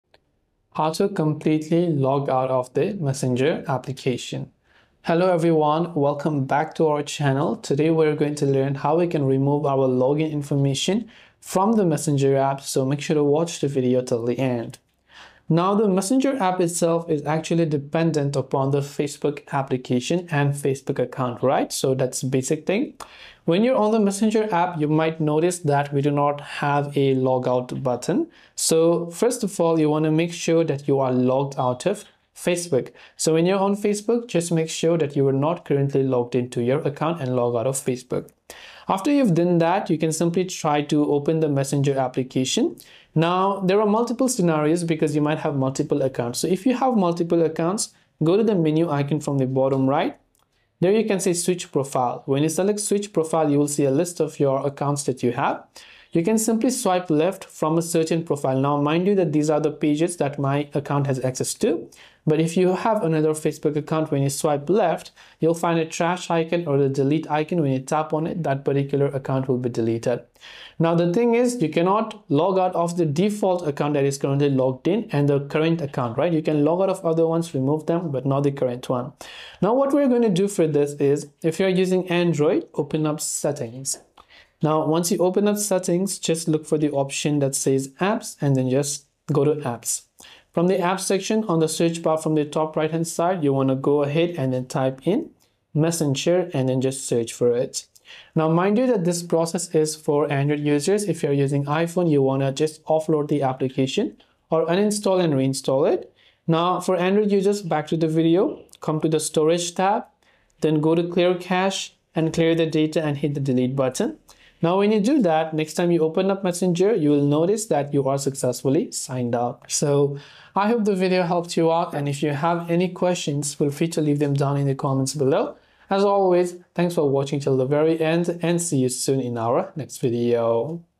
How to Remove Saved Login Info on Messenger | Log Out Completely
If you're wondering how to completely log out of the Messenger app and remove your saved login information, this video offers a clear step-by-step guide. Unlike many apps, Messenger doesn’t provide a simple “log out” button within the app interface, which can make it confusing if you’re trying to sign out or remove your account from the device. This tutorial covers both Android and iPhone users, showing how to ensure you’re fully logged out of Messenger.

Messenger is tied to your Facebook account, so the first step is to log out of Facebook itself. If you're still logged into Facebook, your Messenger app may automatically retain your login session. Open the Facebook app, go to the menu, and log out of your account.

Once you’ve logged out of Facebook, open Messenger. If you have multiple accounts saved in Messenger, tap the menu icon at the bottom right and select "Switch Profile." You’ll see a list of all the accounts currently stored. Swipe left on any account you'd like to remove, and a trash icon will appear. Tap it to delete that particular profile. However, note that you cannot remove the account that’s currently logged in—you’ll need a workaround for that.

For Android users, the next step is to go into your device’s system settings. Navigate to "Apps" or "Application Manager," find Messenger using the search bar, and select it. Tap on "Storage" and then choose both "Clear Cache" and "Clear Data." This will remove all saved login data from the app. When you reopen Messenger, you'll be fully logged out.

If you're using an iPhone, the easiest way to remove login data is by offloading or uninstalling the Messenger app and reinstalling it again. This resets the session and ensures no login info is retained.

Timestamps:
0:00 Introduction
0:24 Logging out of Facebook
0:43 Removing extra Messenger accounts
1:20 Logging out from default account (Android workaround)
1:58 Conclusion